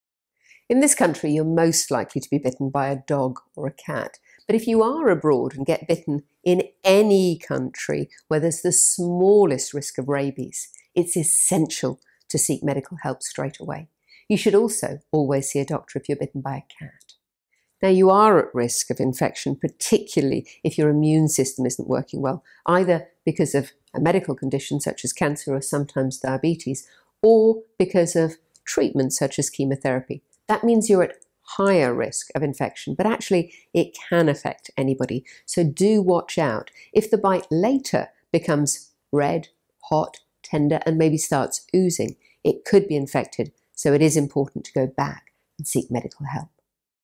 What do I do if I get bitten by an animal?
You should clean the wound no matter how small the cut to the skin. There are many germs (bacteria) in animal mouths. Cleaning will reduce the chance of infection. If the wound is small, you can clean it yourself. Just use ordinary tap water. Let it bleed freely, unless the bleeding is very heavy.